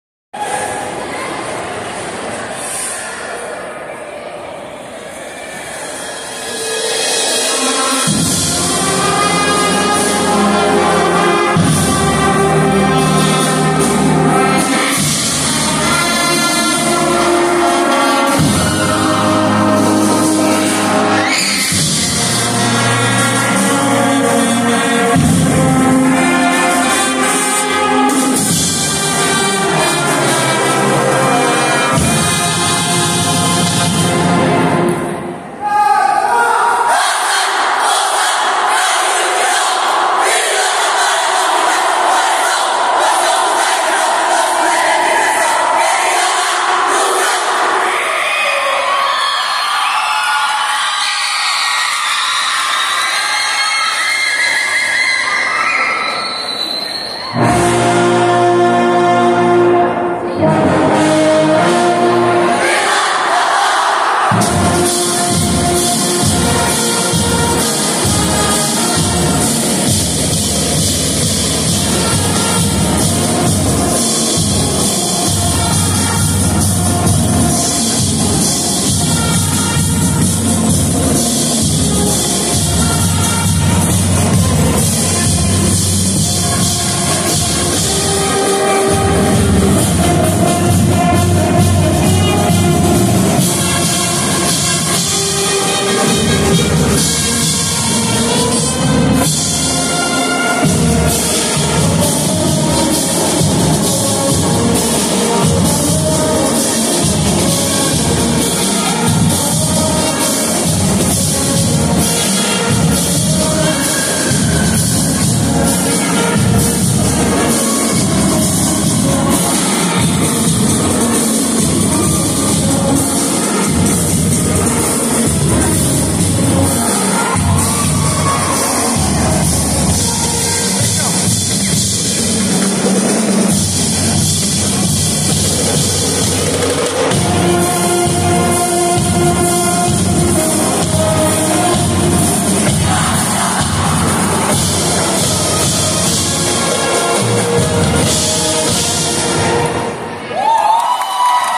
Entrada Banda Marcial Infanto Juvenil Vila da Barra
Banda Marcial Infanto Juvenil Vila da Barra
Entrada Copa Amazônica de Bandas e Fanfarras
Manaus 16/12/23